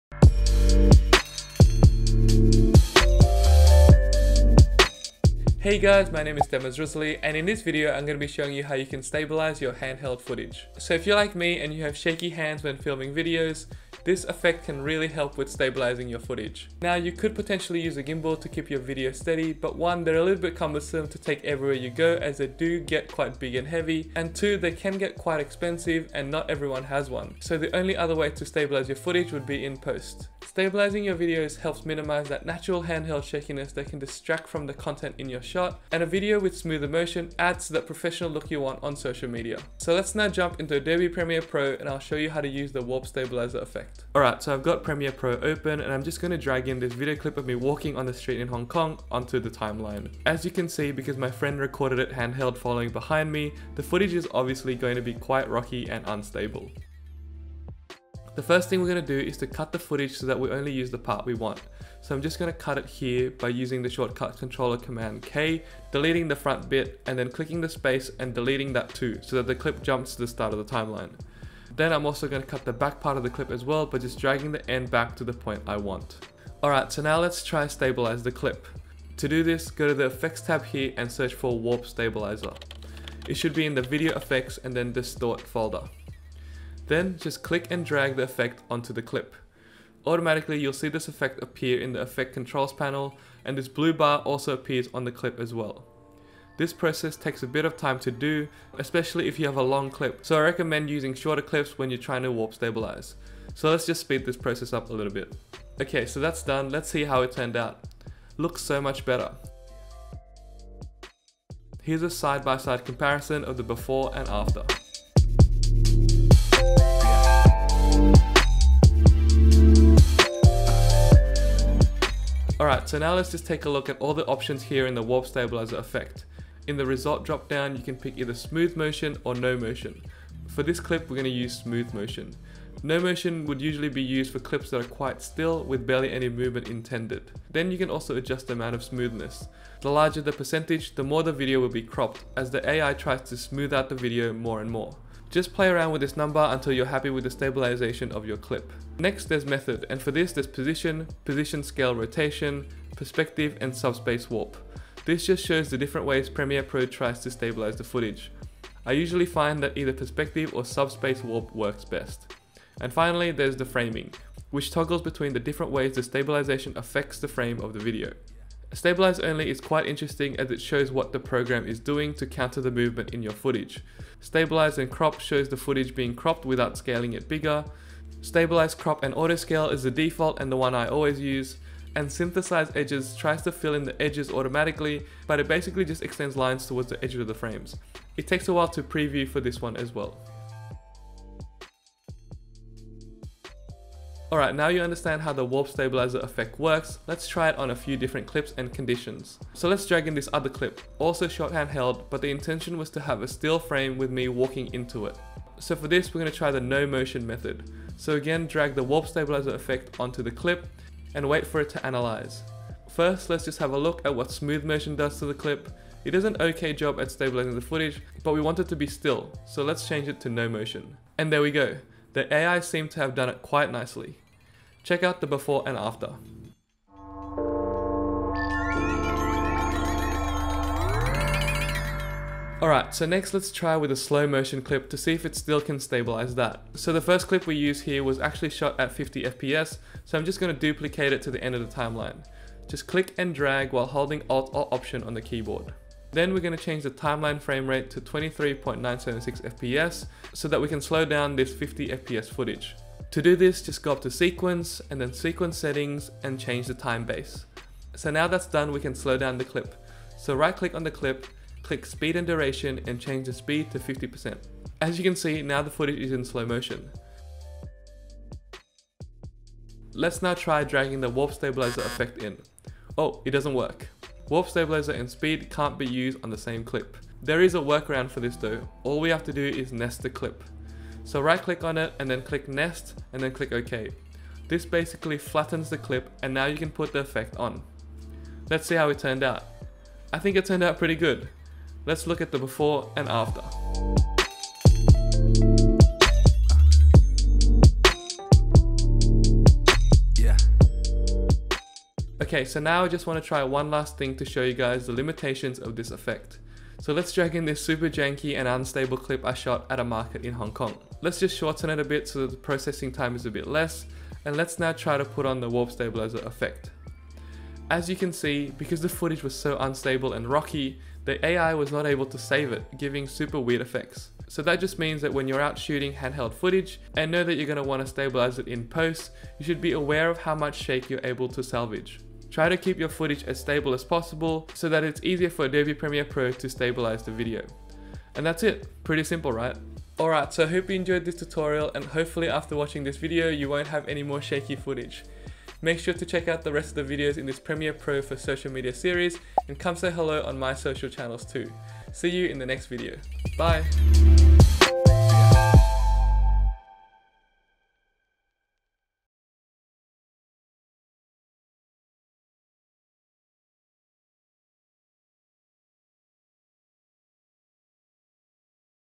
How to Stabilise Handheld Footage with Warp Stabilizer | Adobe Premiere Pro
Learn how to stabilise your handheld footage using the Warp Stabilizer tool in Premiere Pro.​

This Adobe Premiere Pro tutorial @Demas demonstrates how you can stabilise shaky videos with the Warp Stabilizer tool. Use this video stabilization effect to get natural, smoother shots without shakiness that look professional post-edit. Learn how to further fine tune your slow-motion shots with No Motion and Smooth Motion options.
Here are the tips covered in this video:
00:00 Stabilized footage with Warp Stabilizer
2:09 Customize your Warp Stabilizer effect​
2:29 Adjust your stabilization method
3:53 Stabilize slow-motion video clips​
4:37 Use the Nest function to use Warp Stabilizer and Speed together​
5:01 Warp Stabilizer’s limitations​
Want to know more?​
Using Auto Reframe to Create Social Media Videos | Adobe Premiere Pro -